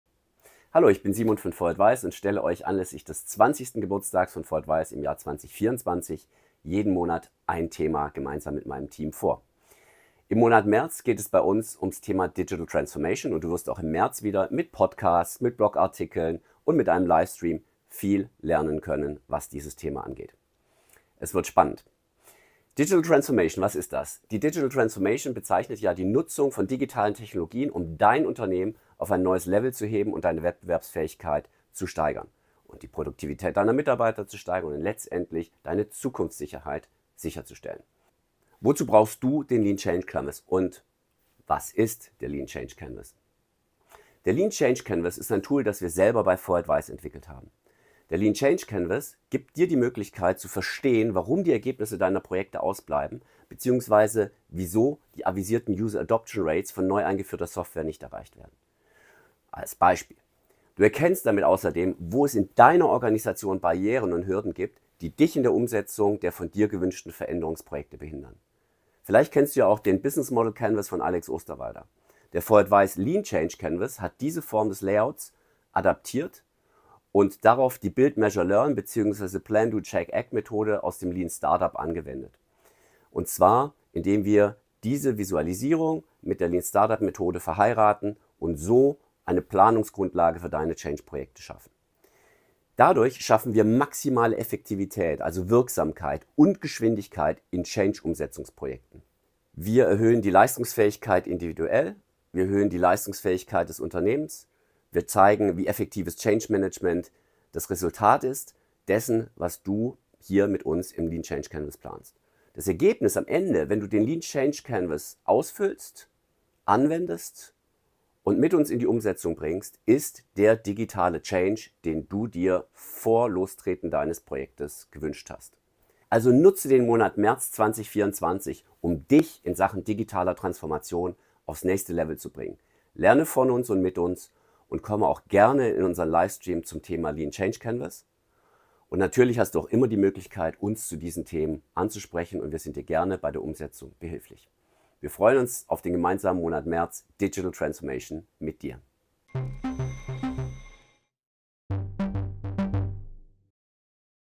Hauptthema März: Digitale Transformation 🚀
Du hörst den Begriff überall, aber was bedeutet „Digitale Transformation" für dich und deine Organisation? Im März tauchen wir mit Podcasts, Blogartikeln und einem Live-Stream tief in dieses spannende Thema ein.
Was ist Digitale Transformation? Kurz gesagt, es geht darum, neue Technologien in deinem Unternehmen einzuführen, um die Wettbewerbsfähigkeit und Produktivität deiner Mitarbeiter zu steigern, sowie die Zukunftssicherheit sicherzustellen.

👉 Die Digitale Transformation ist kein Trend, sondern unerlässlich für neues Wachstum. Sie erhöht die Leistungsfähigkeit, individuell wie auch für dein Unternehmen.

Wir zeigen dir diesen Monat, wie effektives Change-Management funktioniert. Folge uns und gestalte die Zukunft deines Unternehmens aktiv mit!